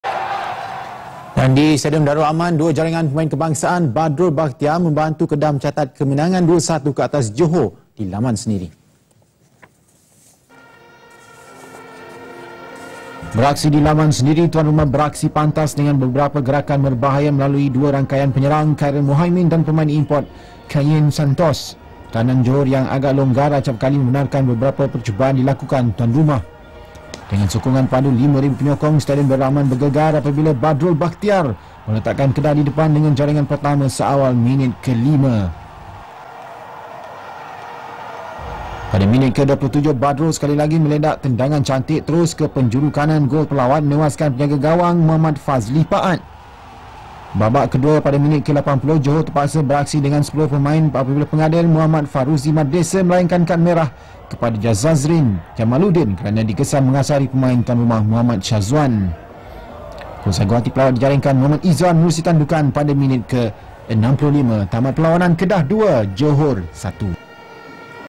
Kedah 2-1 Johor (18.1.2013)
Di Stadium Darul Aman, Alor Setar, Kedah mengalahkan Johor 2-1.